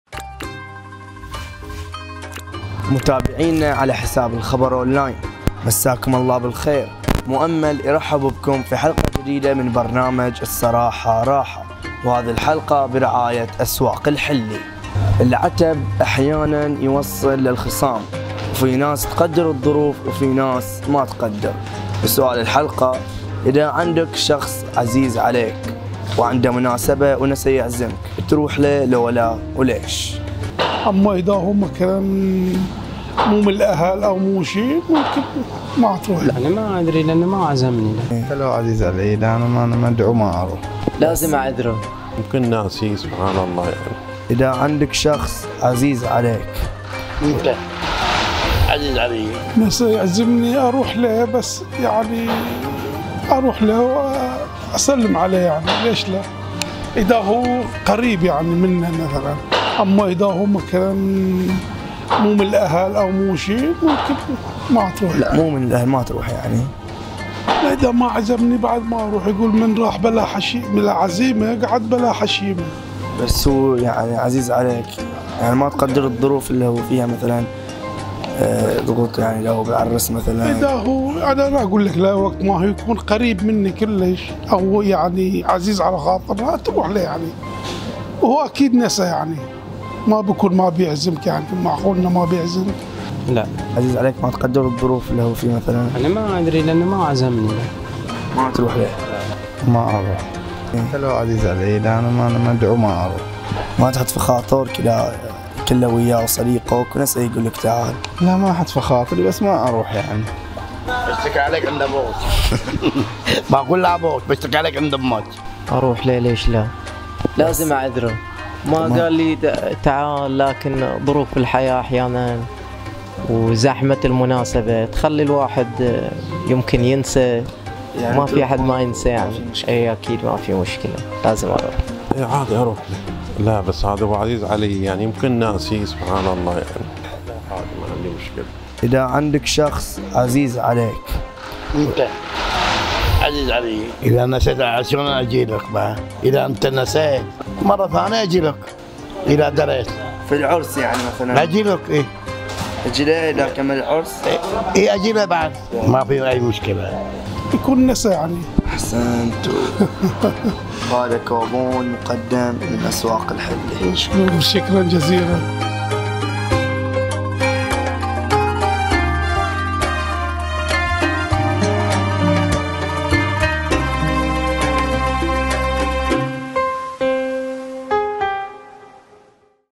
عزيز عليك وما عزمك تروح لو تقول له عافك الخاطر ؟ | الصراحة راحة
مؤمل في حلقة جديدة من برنامج الصراحة راحة، العتب احيانًا يوصل للخصام وفي ناس تقدر الظروف وفي ناس ما تقدر
وسؤال الحلقة : اذا عندك شخص عزيز عليك وعنده مناسبة ونسى يعزمك، تروح ليه لو لا وليش ؟

نرجو الدعم للمزيد من مقاطع الفيديو